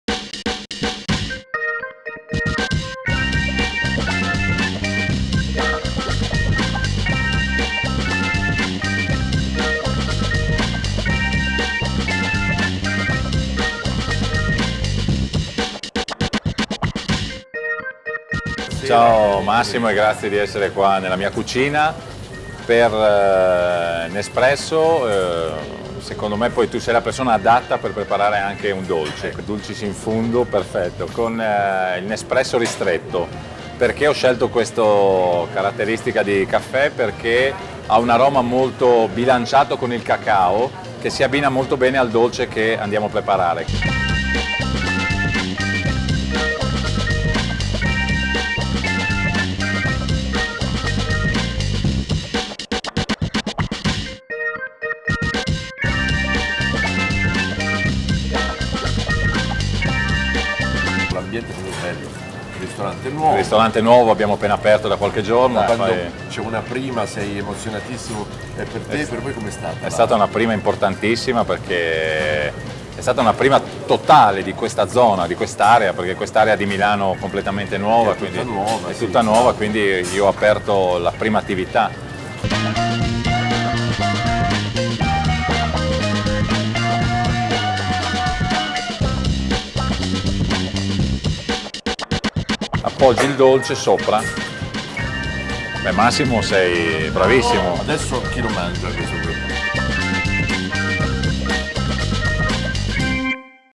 Nespresso Gourmet Experience - Massimo Lopez e Andrea Berton
L'attore Massimo Lopez e lo Chef Andrea Berton preparano il dessert per la Nespresso Gourmet Experience 2013, che si è tenuta presso il nuovo ristorante dello Chef a Milano